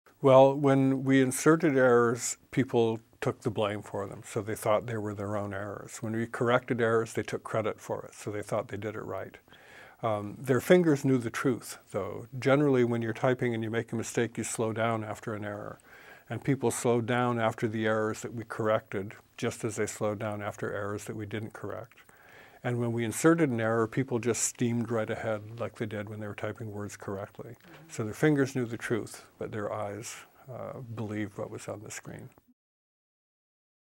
Fingers detect typos
Expert typists are able to zoom across the keyboard without ever thinking about which fingers are pressing the keys. New research from Vanderbilt University reveals that this skill is managed by an autopilot, one that is able to catch errors that can fool our conscious brain.

The research was published in the Oct. 29 issue of Science.

"We all know we do some things on autopilot, from walking to doing familiar tasks like making coffee and, in this study, typing. What we don't know as scientists is how people are able to control their autopilots," Gordon Logan, Centennial Professor of Psychology and lead author of the new research, said. "The remarkable thing we found is that these processes are disassociated. The hands know when the hands make an error, even when the mind does not."

Watch Gordon Logan discussing his research.